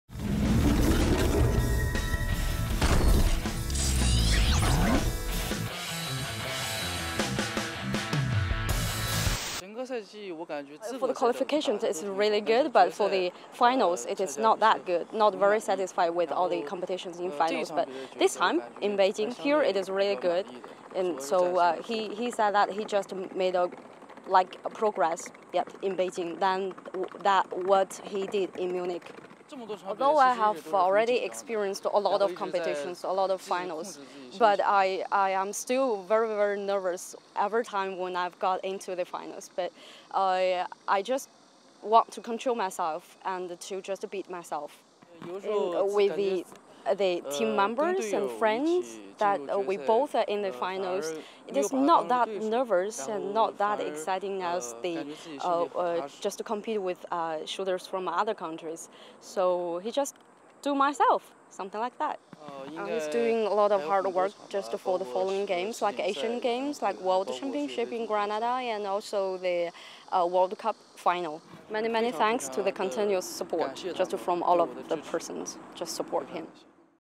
Interview (ENG) with Zhiwei WANG (CHN) - ISSF World Cup in all events 2014, Beijing (CHN)
Interview - The 2012 Olympic Bronze medallist of China beat the reigning Olympic Champion Jin of Korea, to win the 50m Pistol Men final in front of his teammates and supporters at Beijing's shooting range.